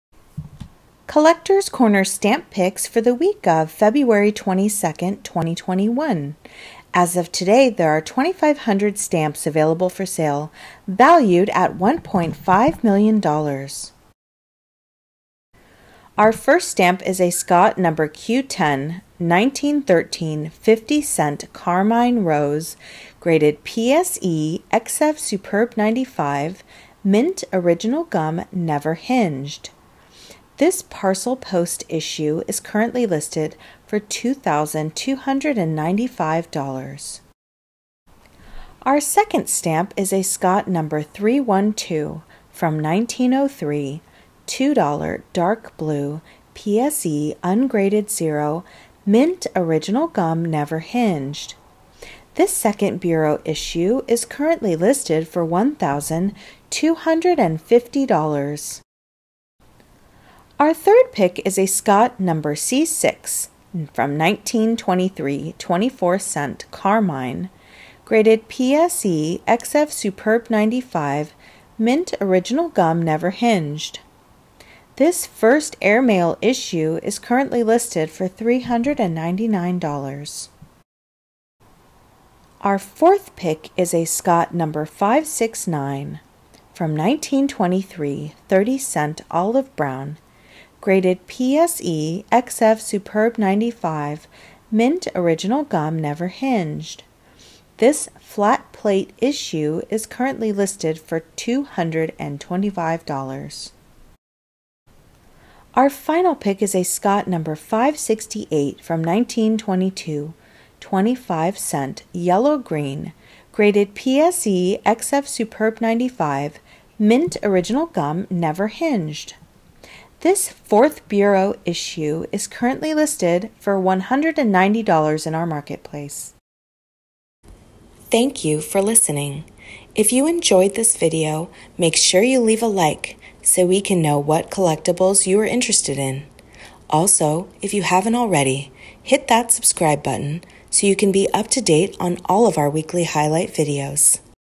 Collectors Corner: Weekly Stamp Picks – Week of February 22, 2021
If you are interested in viewing or purchasing any of these stamps, go Your stamp is just around the corner at Collectors Corner!